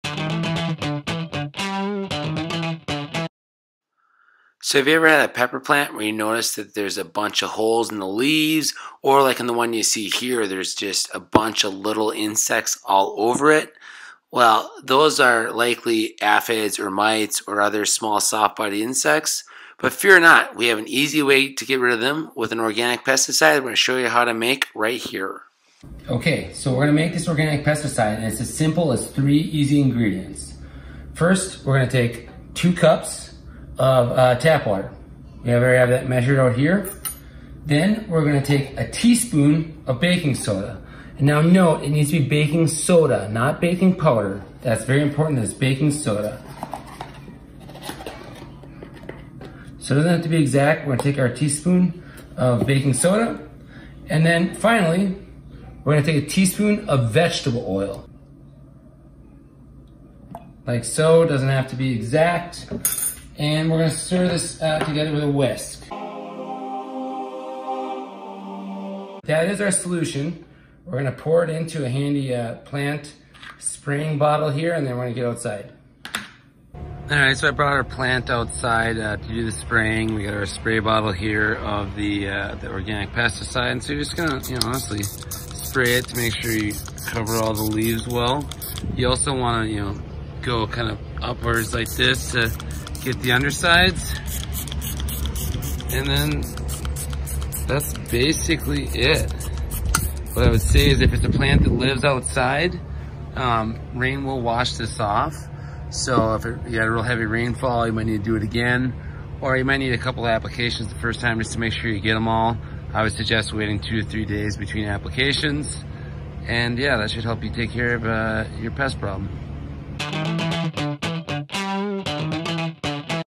Stop Bugs From Eating Your Pepper Plants! Easy to Make Organic Pesticide.
Easy Organic Pesticide that is effective against Aphids, Mites, and other pests.  Link to a highly rated plant spray bottle for applying pesticide

This Organic Pesticide is made from baking soda, vegetable oil, and water.